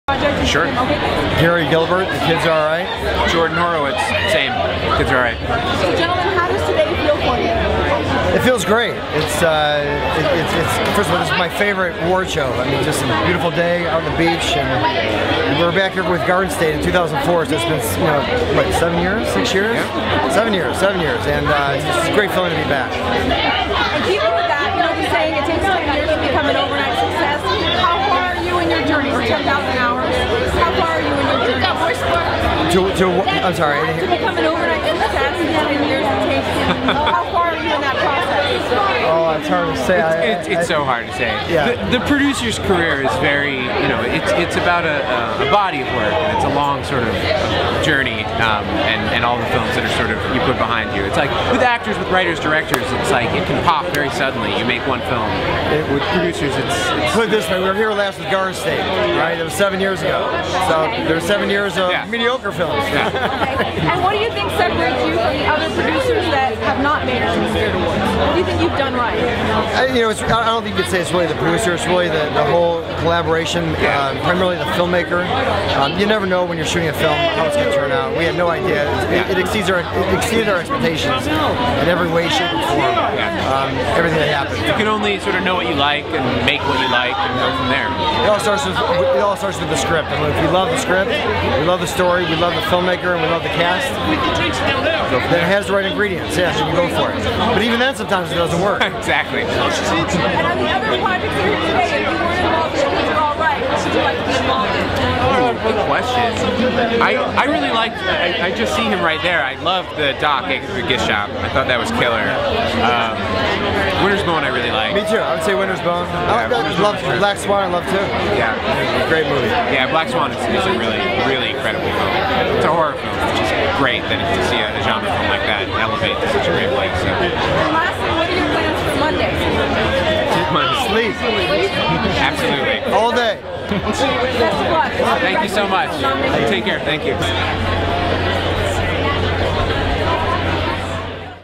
Gary Gilbert & Jordan Horowitz at 26th Film Independent Spirit Awards (2011)
Producers Gary Gilbert & Jordan Horowitz on the red carpet of the 26th Film Independent Spirit Awards (2011). Their film "The Kids Are All Right" is nominated for Best Feature, Best Screenplay, Best Director, Best Female Lead, & Best Supporting Male.